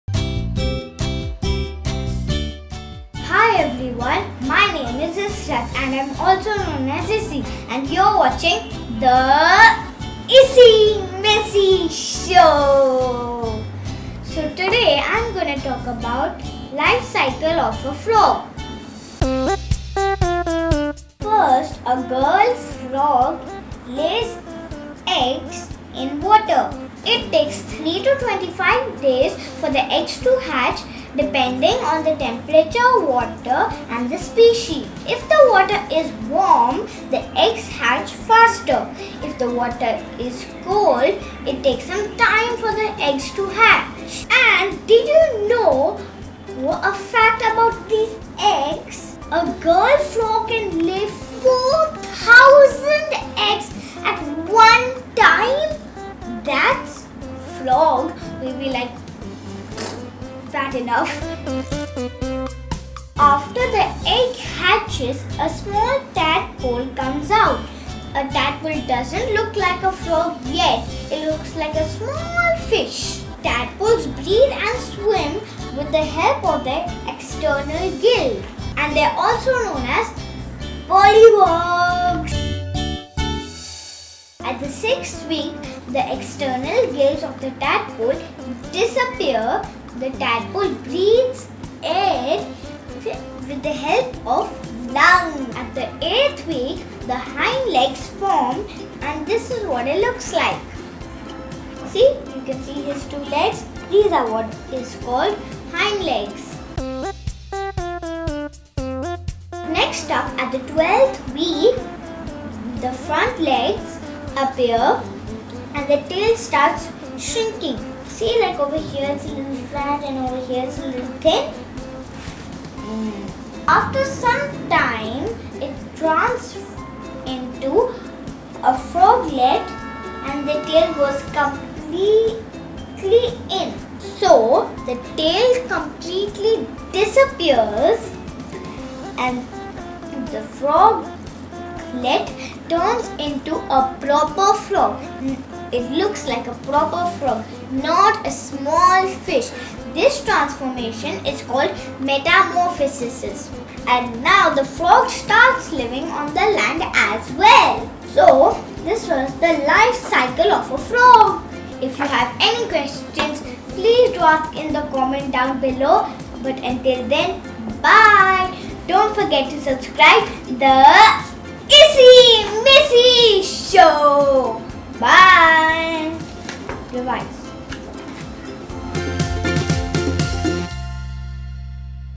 Frog Life Cycle - From Egg to Frog | Science for Kids
Frogs are fascinating creatures for young children. Learn interesting facts about the life cycle of a frog with Israt. What is a life cycle? A life cycle refers to the changes an animal goes through while it's alive. Also, frogs go through several stages of life before they become adult frogs and during those stages, they live only in water.

-Play Doh For Kids | Learn Colors And Shapes Playset | Color Learning Video With The Issy Missy Show